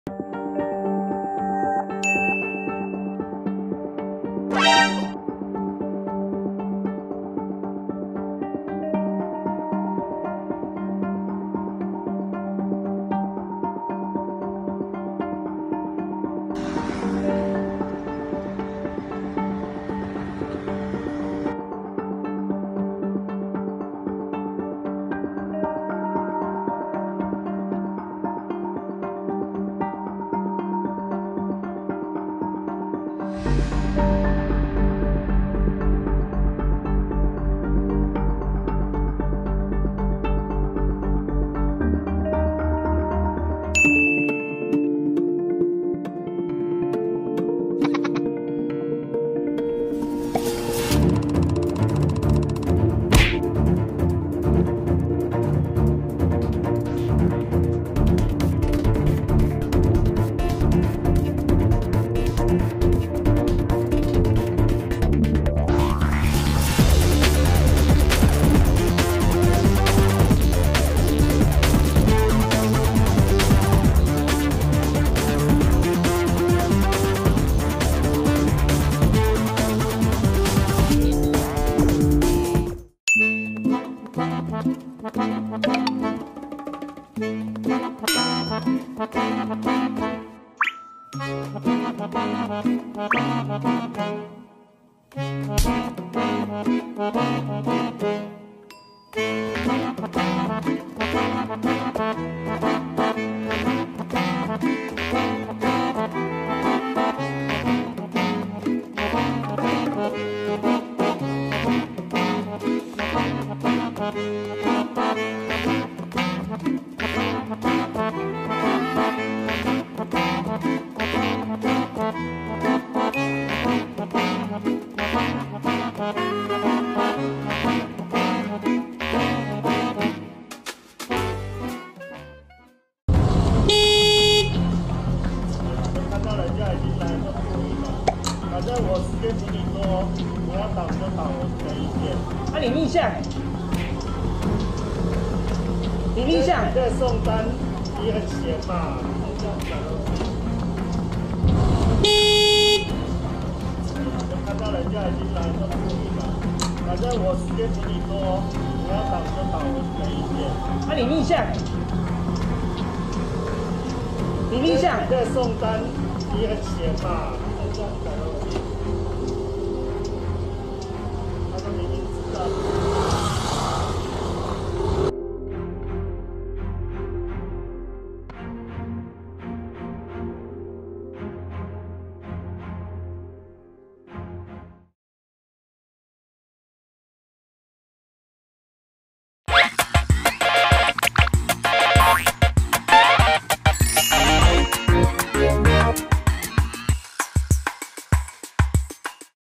女騎士馬路「鬼切迴轉」 砂石車視線死角起步就擊落｜車禍｜違規｜社會
☑影片來源：民眾、八德警分局 提供